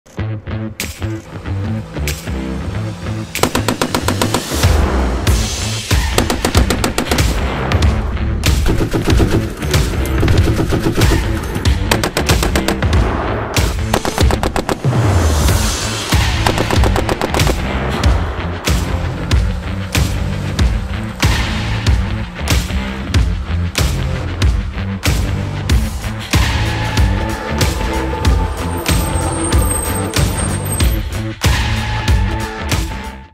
Get the Strykers on the range!
2nd Battalion, 1st Infantry Regiment "Legion" Division conduct gunnery training with Stryker at Rodriguez Live fire Complex (RLFC), South Korea on 09 Feburury 2023.

📹 by Songho Yun